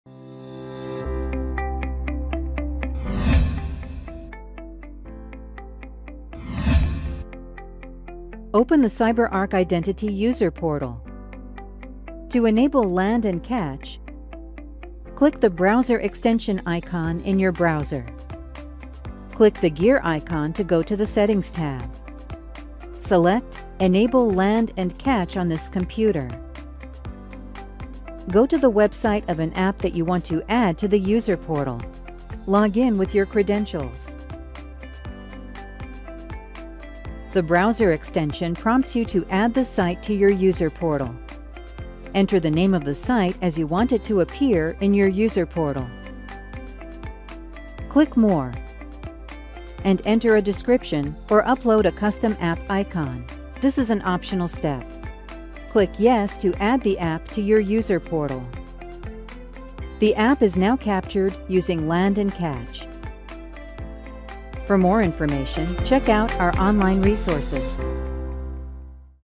Workforce Password Management Land and Catch
Workforce Password Management (WPM) Land and Catch feature automatically recognize when users enter credentials and offers to add new apps to the user portal for convenient access. Securely auto-fill credentials fields for a smooth and quick login experience.